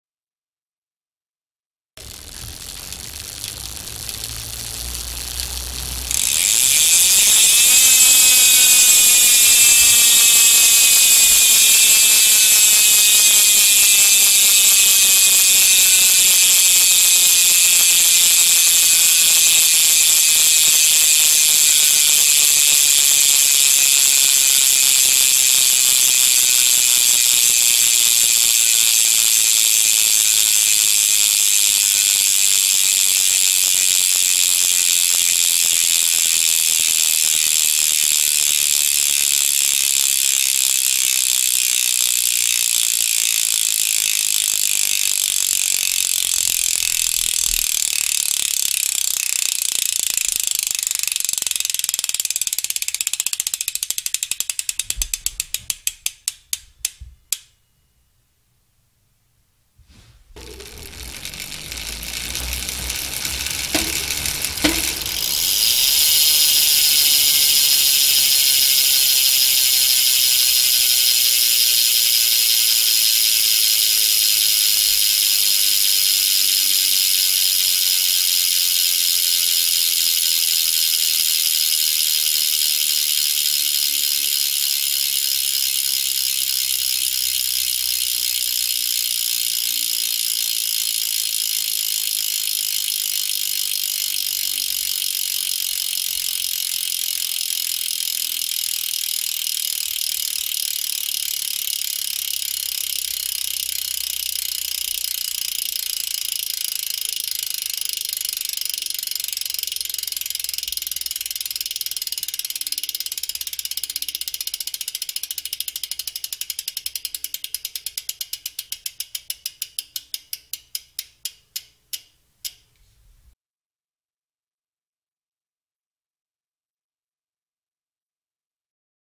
Hope Tech Pro 4 Hub Sound
This is a bit of a ridiculous video to make but when I was considering buying a Hope Tech Pro 4 Hub I really wanted to have an accurate idea of what they sounded like. It bugged me that the existing recordings were done based on a camera's built-in mic because that automatically transforms the audio and picks up a lot of room noise. This recording is made using an Audio Technica ATR2100 supported in a mic stand about 12 inches from the hub.

For comparison purposes I recorded both the Pro 4 hub and a Bontrager Duster Elite hub with the exact same setup, back-to-back. Each wheel was attached to my YT Jeffsy 27, though the wheel with the Pro 4 hub has a Specialized Slaughter on it while the wheel with the Duster Elite has a Bontrager XR4 on it.  This is about as fair of a comparison as I could make.